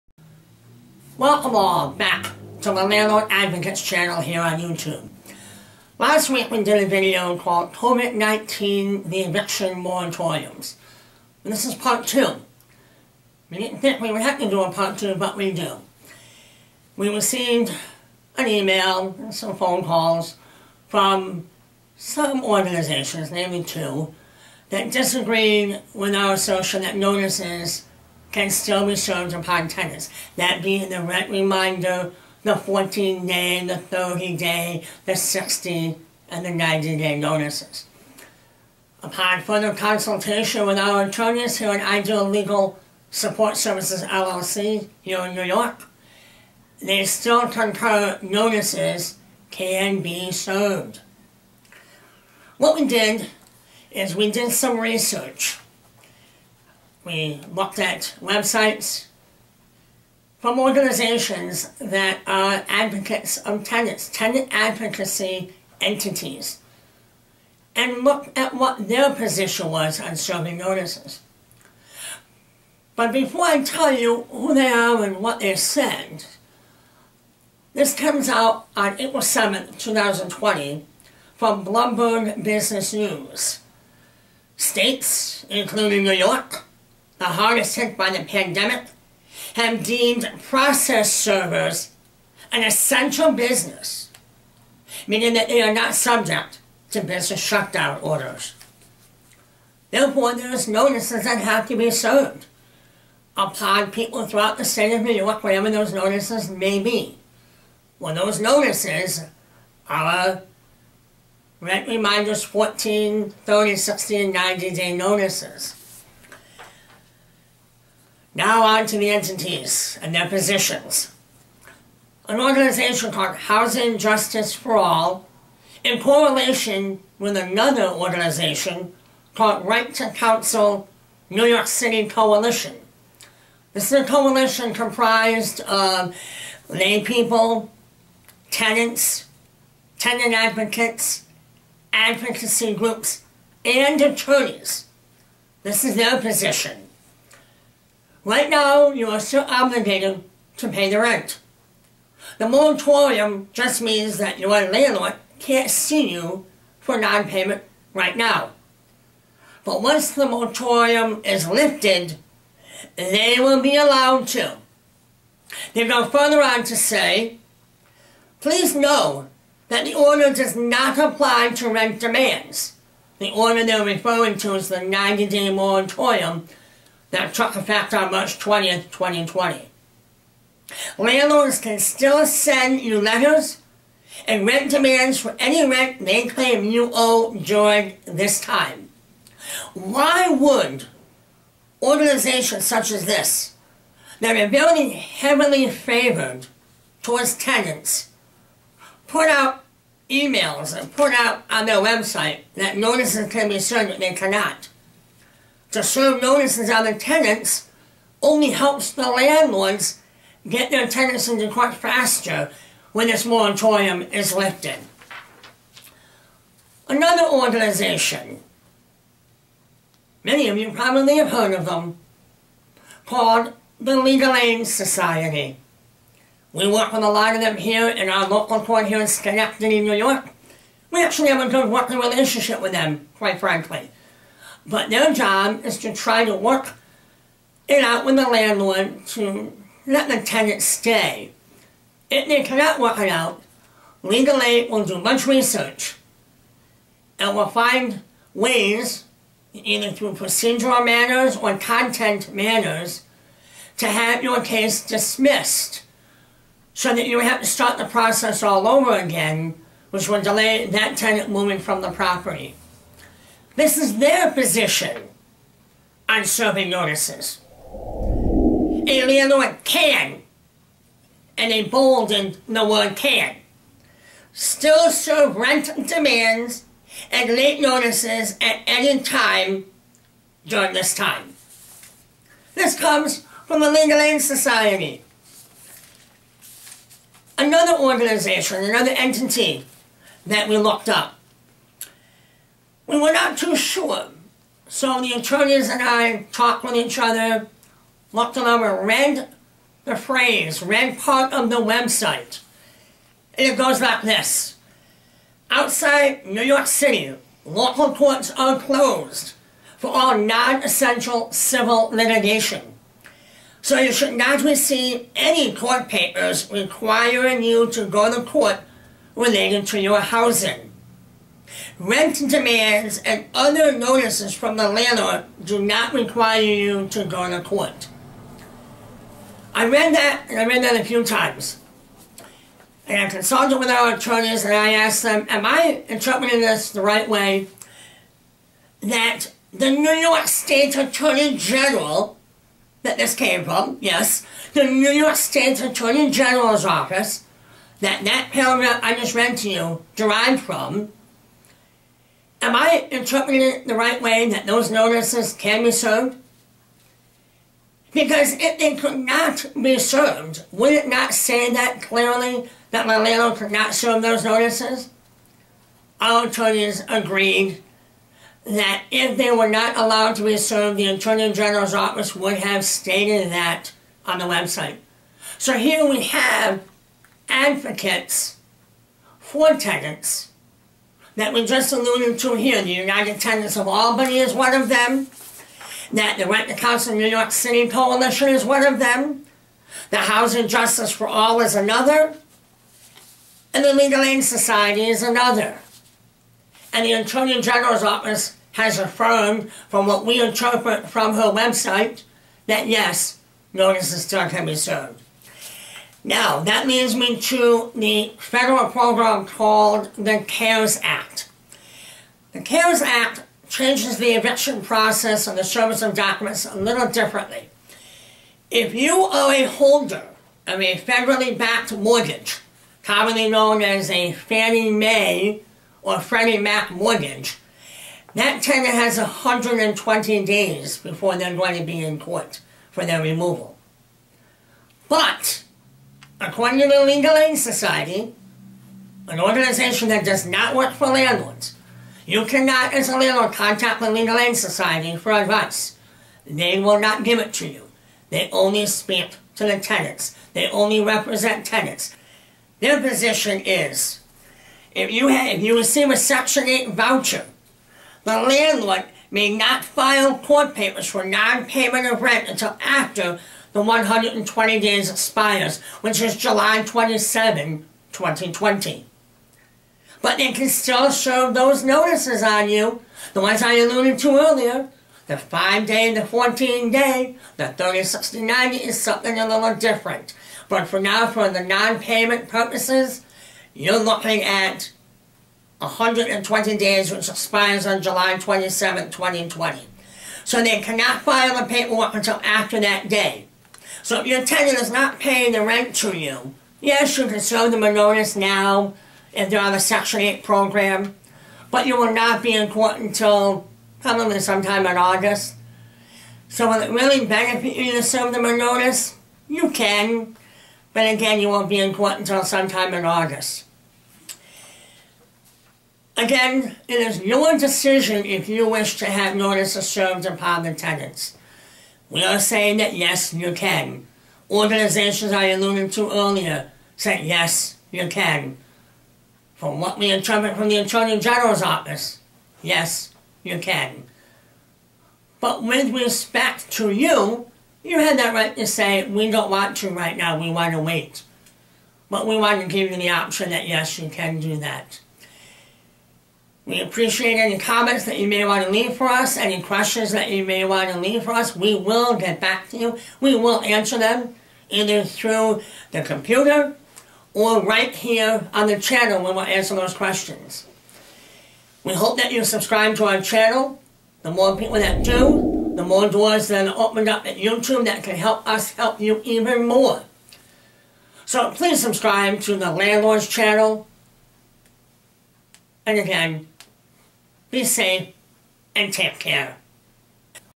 DO I STILL HAVE TO PAY MY RENT DURING THE EVICTION MORATORIUM????
Do I still have to pay my rent during the eviction moratorium?  Our continuing discussion on serving documents during the 90 day moratorium in New York. We had a few organizations and individuals who disagreed with our stand that notices could still be served upon tenants.  We felt that we needed to acquire other references to substantiate our claim.  In this video we have mentioned a few.  NOTE: Two days after publication of this video, those same organizations who disagreed with us, distributed information that we were in fact, CORRECT!!!  Notices can be served upon your tenants here in New York State!  The 90 day moratorium was extended by Governor Cuomo through August 20, 2020.